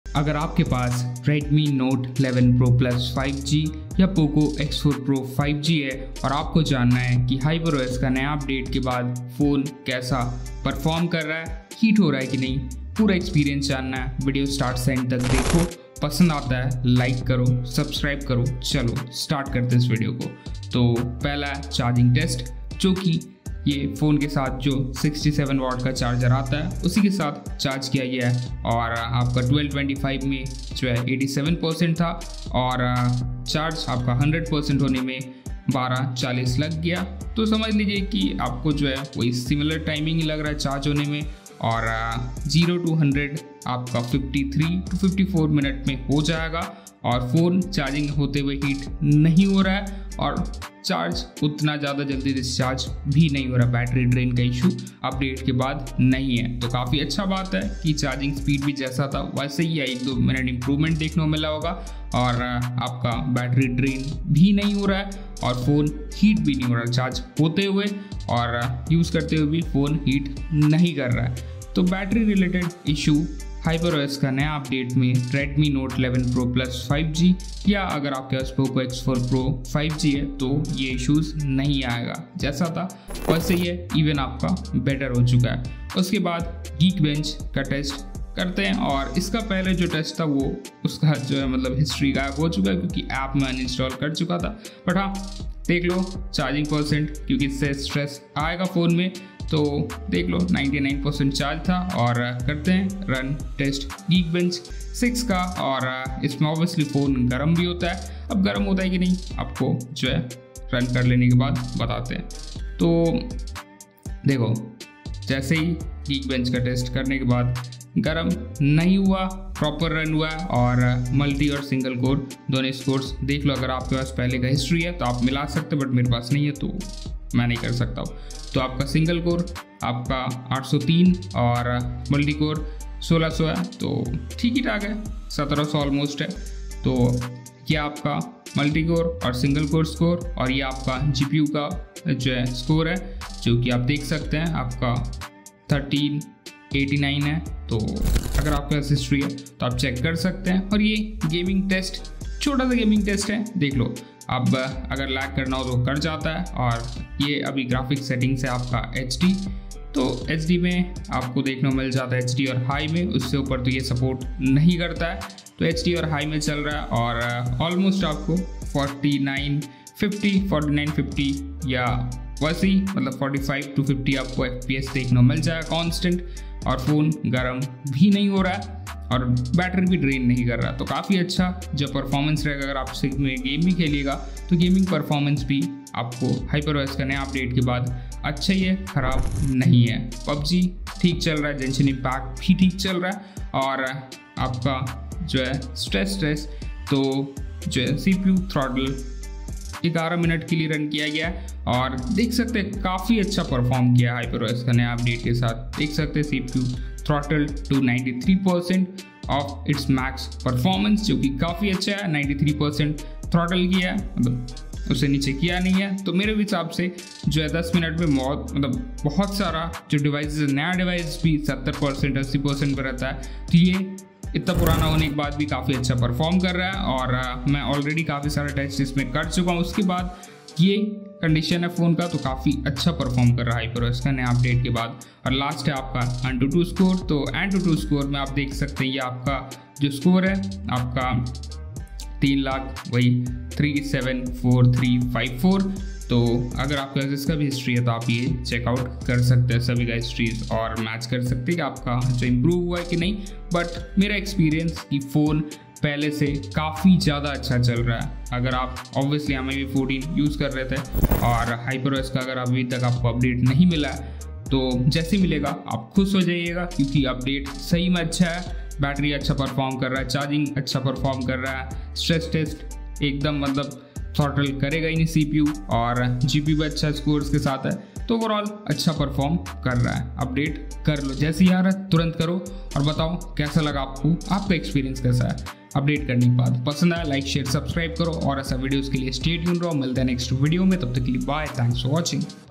Redmi Note 11 Pro+/Poco X4 Pro Hyper Os 1.0.1.0 Performance,Charging,Gaming Test & More
Background Music 🎶🎵🎵🎵.